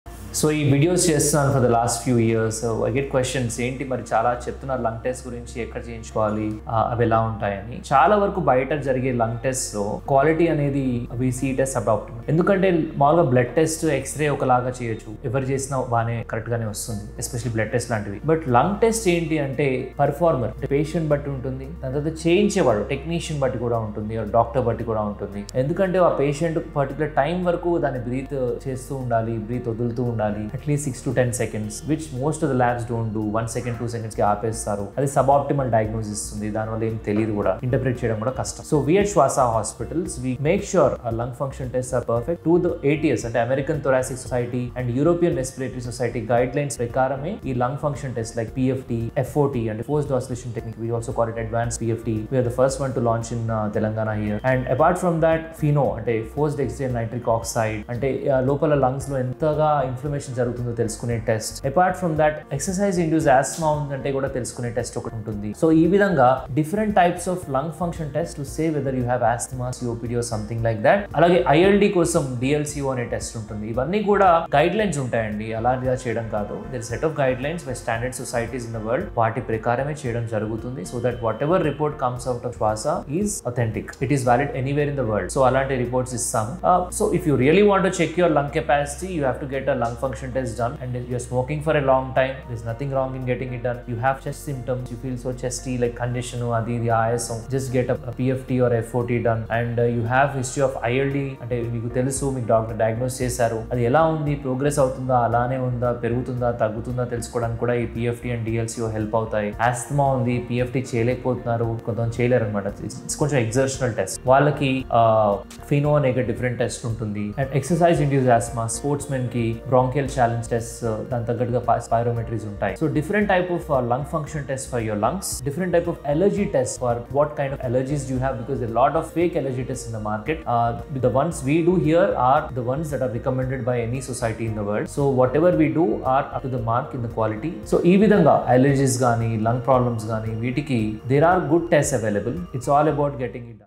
Getting Your Lungs Tested – Do It Right
Are you getting accurate lung function tests? Here's what most people miss.